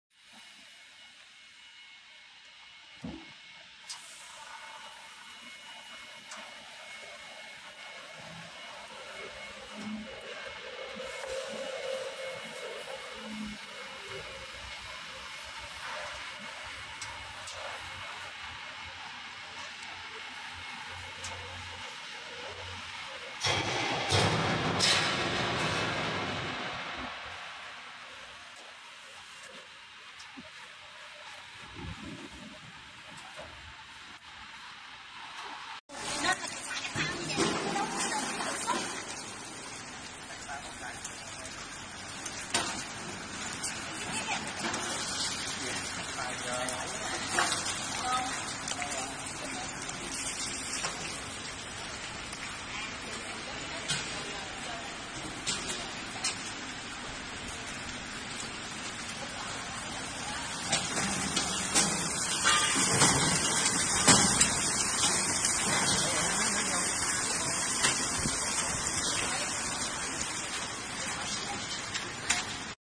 Conveyor Mesh Belt Frying Machine | Continuous Chips Fryer Machine
Continuous fryers are known for their excellent performance and fast and efficient frying capabilities. Through a sophisticated control system, they ensure that food is crispy and delicious while maintaining the optimal cooking temperature. These designs bring a safer and smarter use experience.

Please contact CARRY to get more details